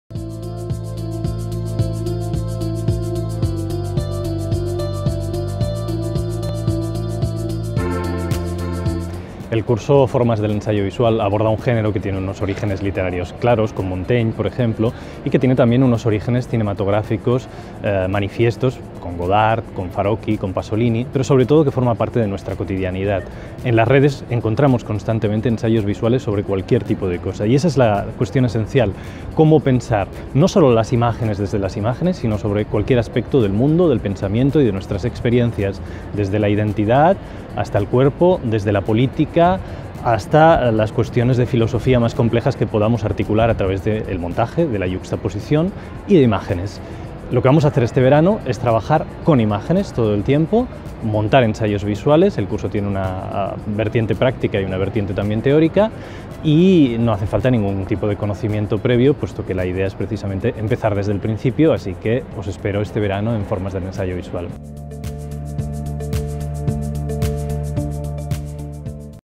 Formas de ensayo audiovisual - BISS
A partir de la figura central del padre de la iconología, Aby Warburg, y de la inscripción del estudio de la imagen en un marco más amplio, la “ciencia de la cultura”, el curso plantea un acercamiento dinámico a las metodologías del pensamiento en imágenes y el ensayo visual. El curso pretende hacer del pensamiento en imágenes una forma transdisciplinar de abordar otros campos expresivos y formas de reflexión.

Por una parte, se trata de acercarse a la creación de imágenes desde el gesto y en función de las lógicas del corte, el montaje y la yuxtaposición. Por otra, y de acuerdo con una metodología de proyecto, se trata de activar la idea de una imagen capaz de provocar choques sobre las otras imágenes para estimular una lógica creativa fundamentada en el control de las supervivencias de la imagen, en su dimensión gestual  y en las fórmulas afectivas de pathos.

Cada sesión del curso, conforme a un doble eje teórico y práctico, intenta proporcionar herramientas para explorar las posibilidades de escritura que ofrece el ensayo visual –ritmo, tensión, continuidad, yuxtaposición, utilización de la voz y del texto-, al mismo tiempo que invita a abordar otras formas visuales contemporáneas –cine, ficción televisiva, instalaciones artísticas, cómic, videoclip, apps-, de manera arqueológica: a través del pensar háptico que ofrece el montaje, el ensayo visual se convierte en un modo de explorar supervivencias pictóricas, escultóricas, literarias y coreográficas.

El gesto, la danza, los ritmos del cuerpo, la temporalidad, el marco, la secuencia o las formas epistolares son algunos de los caminos a través de los que se trata de preguntarse ¿En qué piensan las imágenes contemporáneas?¿Cómo nos piensan las imágenes que vemos? ¿Qué sucede en el observador?¿Cómo nos podemos acercar a la vida en movimiento de las imágenes para tratar de desvelar otras maneras de pensar?